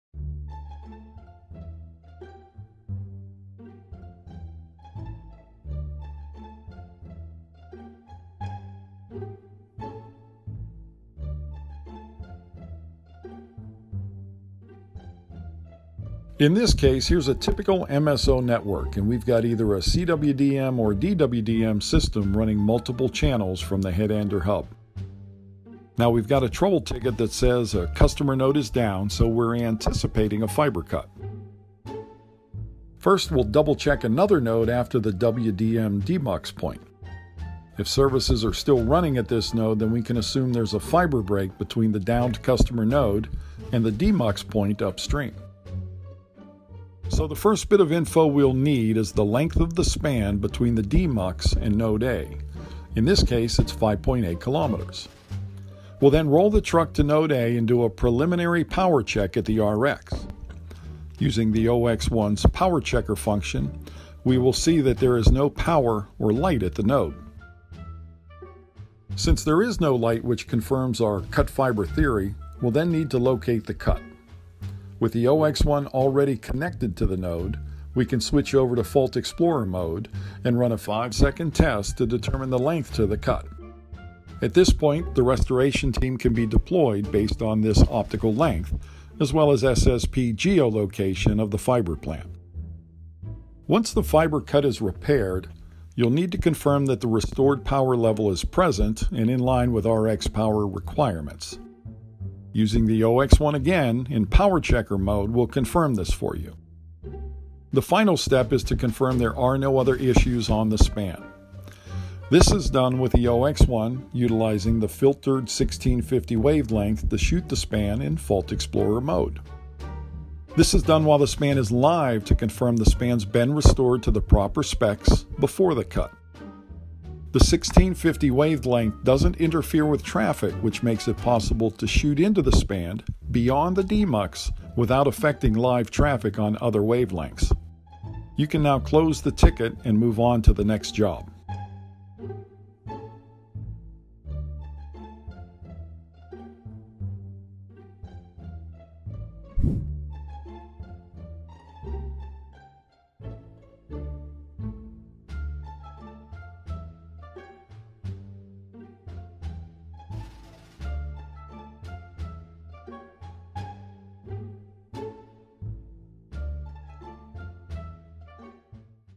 How to troubleshoot a fiber optic cable cut - Use case
Learn all about different network troubleshooting use cases presented in  "The MSO chapters". This specific video explores a fiber cut scenario into a Hybrid Fibre Coaxial (HFC) network, where a technician uses EXFO's Optical Explorer to determine distance to break, properly repairs the fiber, and ensures power levels are restored.